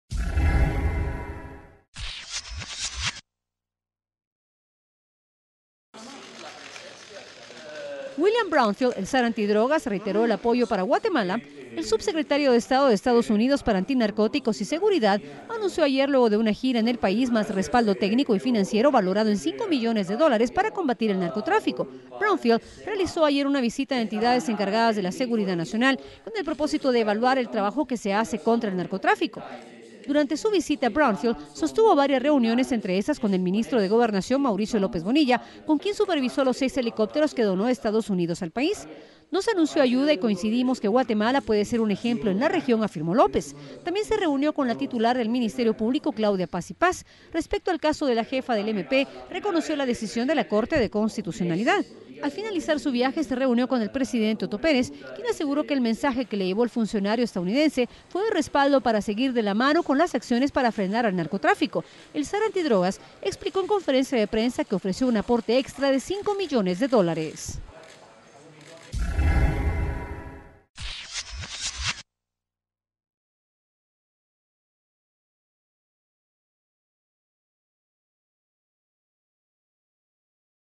E.E.U.U. respaldará lucha contra narcotráfico
Brownfield, el "Zar" antidrogas reiteró el apoyo a Guatemala contra el narcotráfico con más respaldo técnico y financiero valorado en cinco millones de dólares